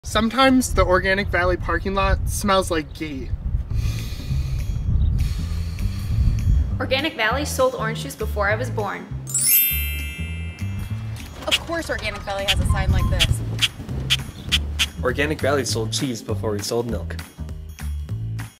Organic Valley Secrets | Part 1
Organic Valley Secrets: Organic Valley started in 1988 as just seven farmers. Our founders had the crazy idea that food could be raised the right way, and farmers should be treated fairly. It’s an idea as radical today as it was back then. More